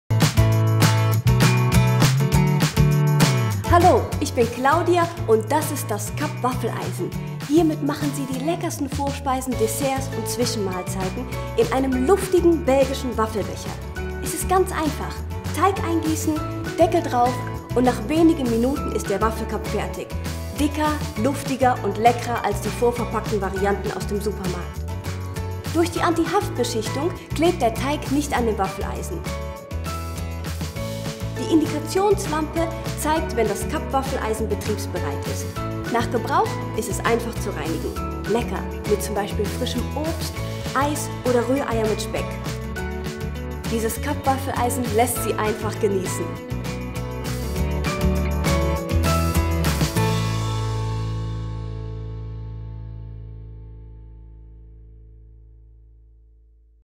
EMERIO WAFELCUPMAKER WBM-110196 GER HD
Das Emerio Cup Waffeleisen. Hiermit machen Sie die leckersten Vorspeisen, Desserts und Zwischenmahlzeiten in einem knusprigen  luftigen/softem Belgischen Waffel Becher. Lecker mit zum Beispiel frischem Obst, Eis oder Rühreier mit Speck.